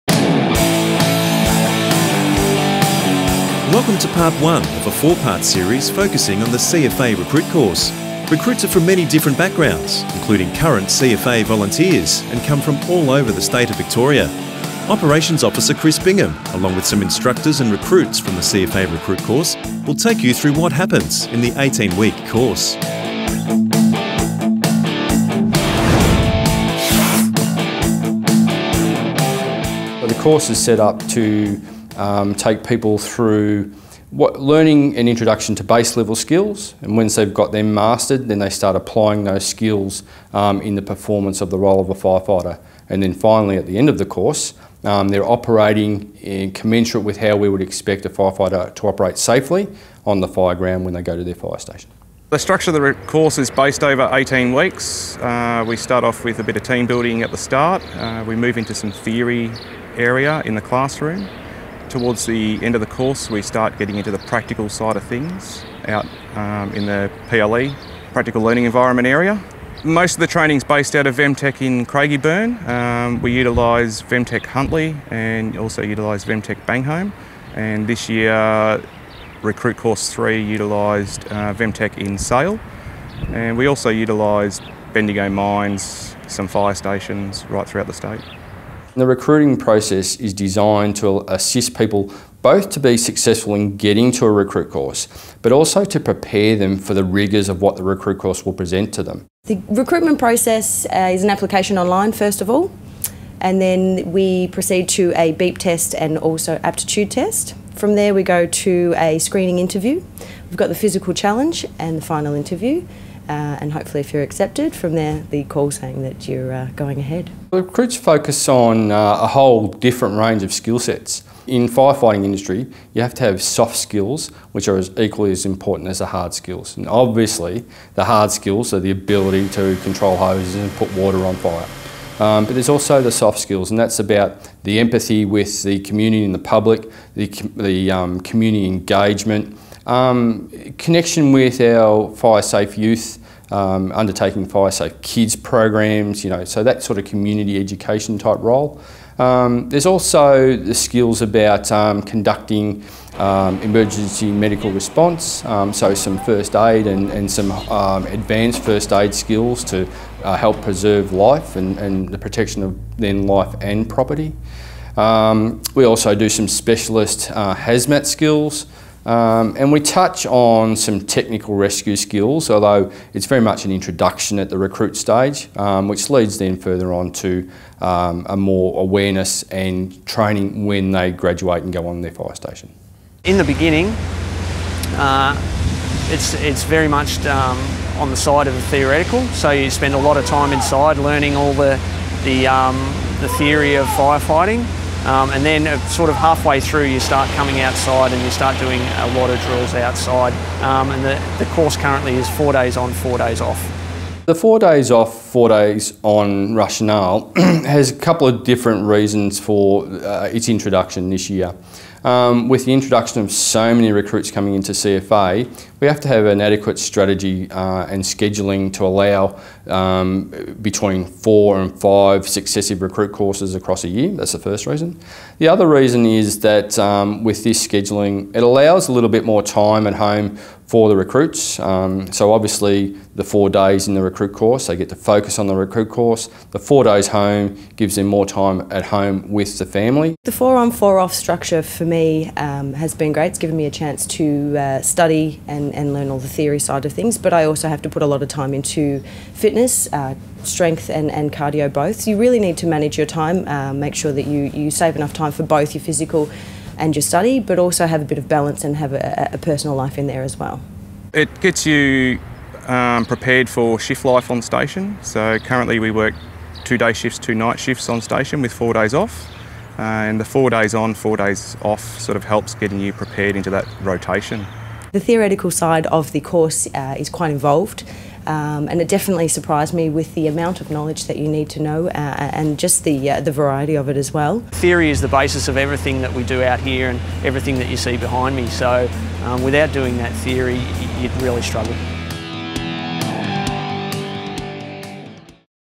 CFA Recruits Episode 1 - 2016
We are looking for men and women who are interested in becoming a career firefighter with CFA and have created this 4 episode series showing you what can be expected during the course.